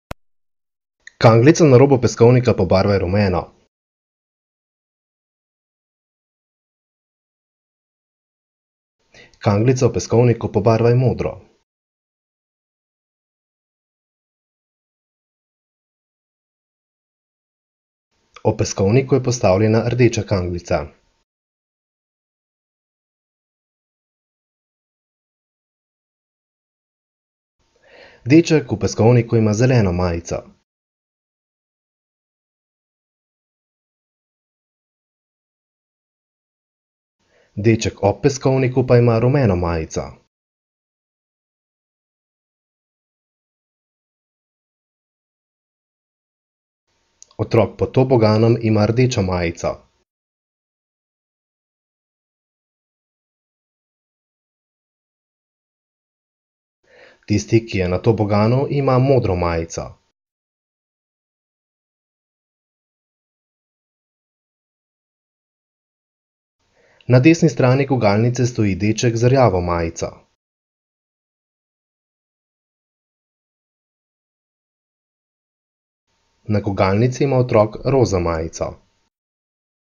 NA IGRIŠČU
S slikanico na rami 1, delovni zvezek za jezik in književnost, str. 46
Bere: Klemen Mišič